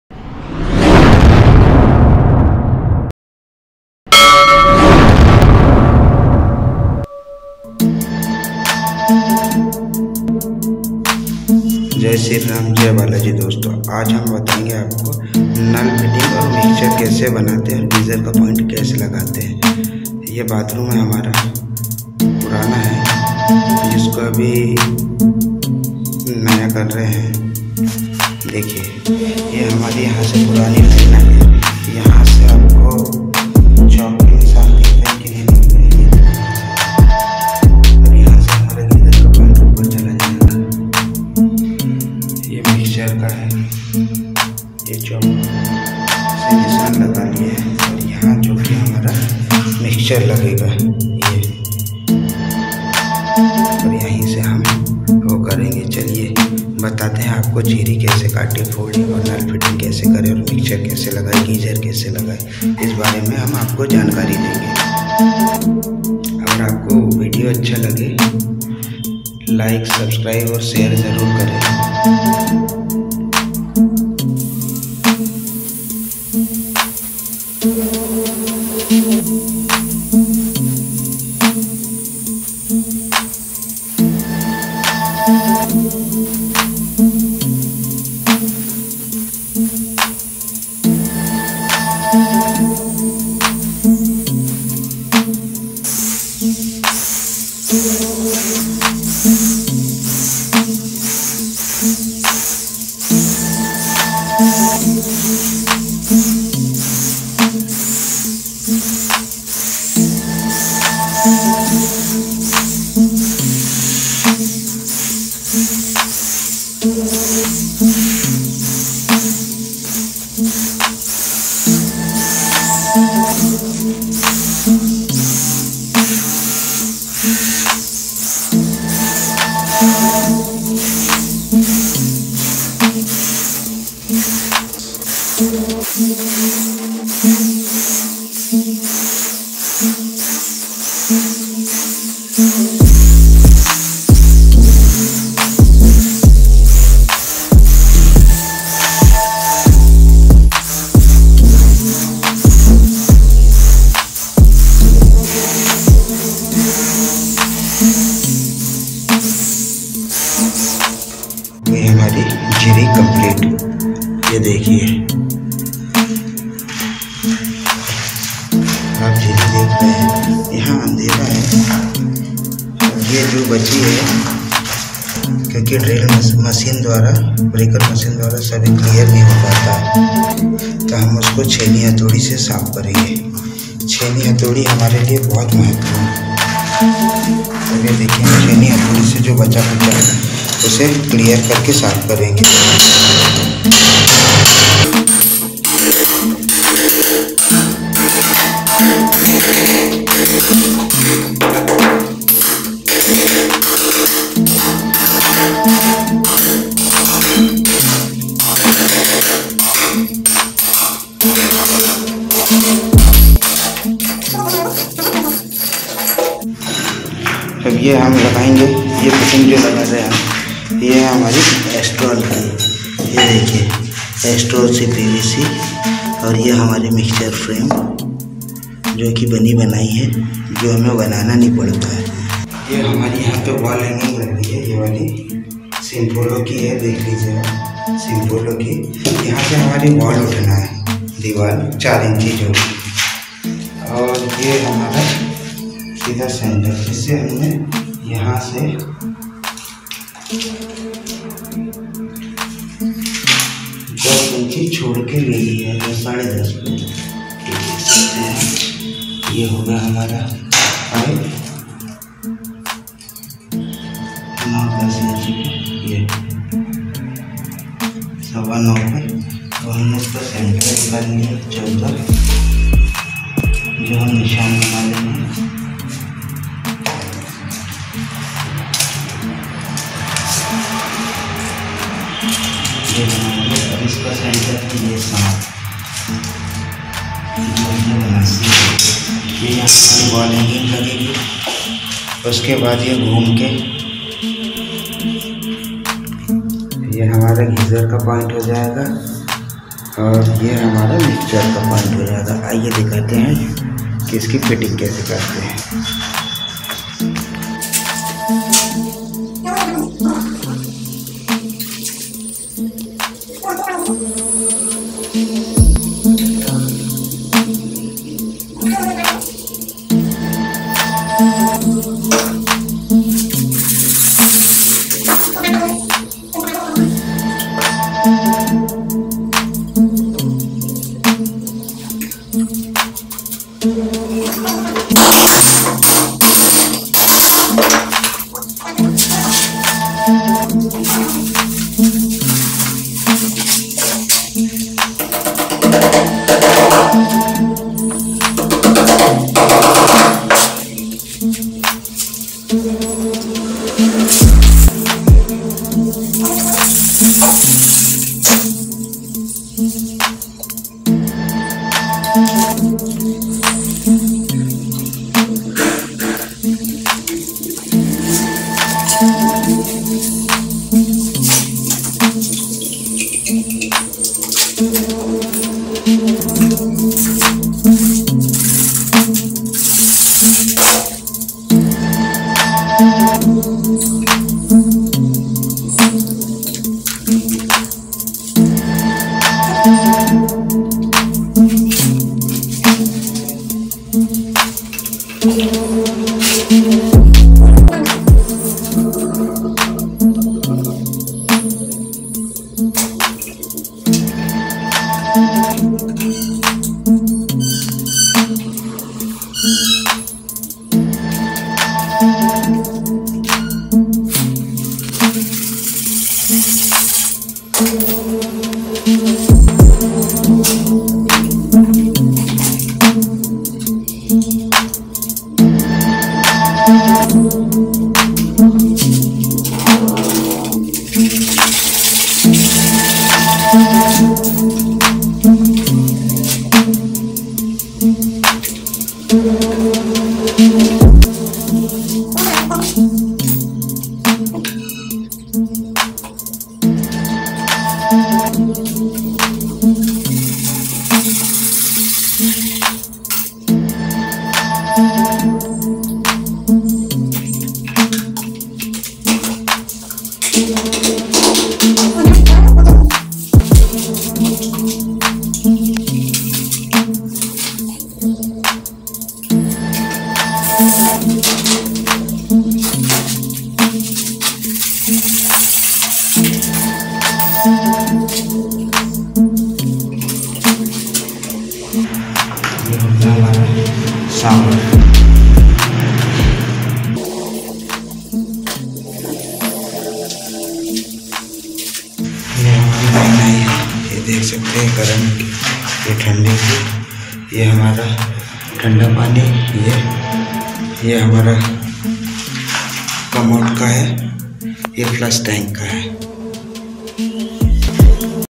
Hot and cool water mixture fiting
Hot and cool water mixture firing
one pish commode lagana sikhe
Ramji ki doston
            main is video mein one piece commode lagane ke bare mein bataya hai